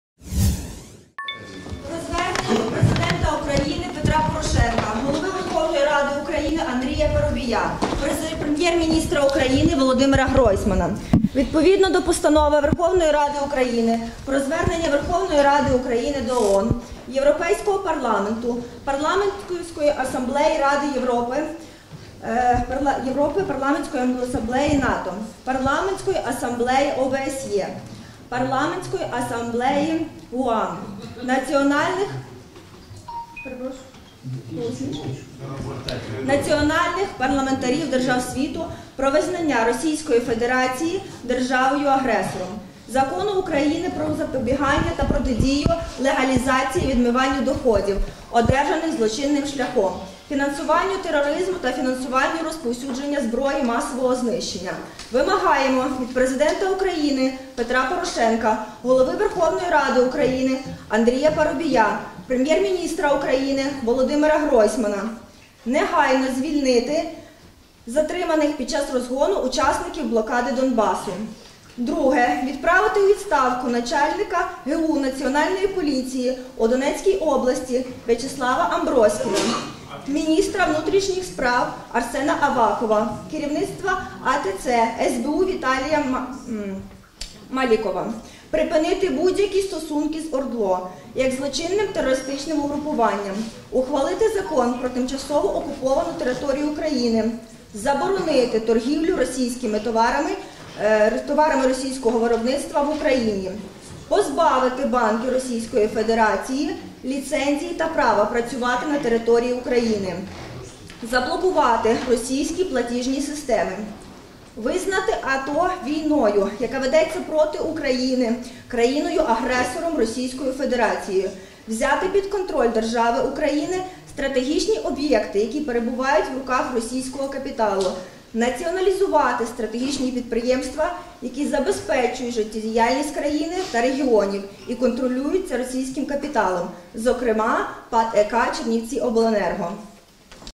Депутати ЧМР звернулися до Порошенка щодо недопущення торгівлі з ОРДЛО
Чернівецька міська рада під час екстреної позачергової сесії у ніч проти вівторка ухвалила звернення до Президента України Петра Порошенка, Голови Верховної Ради Андрія Парубія, Прем’єр-міністра Володимира Гройсмана.

Депутати, зокрема, вимагають негайно звільнити затриманих під час розгону учасників блокади Донбасу, а також відправити у відставку начальника ГУ Нацполіції у Донецькій області В’ячеслава Амброськіна, міністра внутрішніх справ Арсена Авакова, керівника АТЦ СБУ Віталія Малікова.

«Припинити будь-які стосунки з ОРДЛО як злочинним терористичним угрупуванням, ухваливши закон «Про тимчасову окуповану територію України».
Заборонити торгівлю товарами російського виробництва в Україні.
Позбавити банки Російської Федерації ліцензії та права працювати на території України, заблокувати російські платіжні системи», – ідеться у зверненні.

Новини Чернівці Коментарі - відео телеканалу Чернівецький Промінь (с) 170314